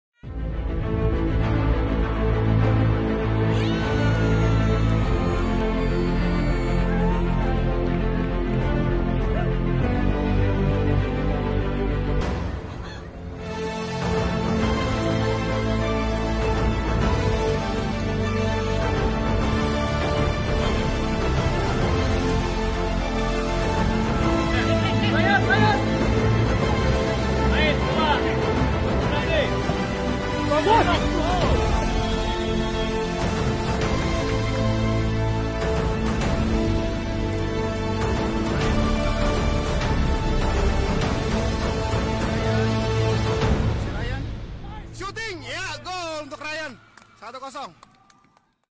Gol Ryan Halim Handoko
Gol Ryan dr jarak jauh di beberapa turnamen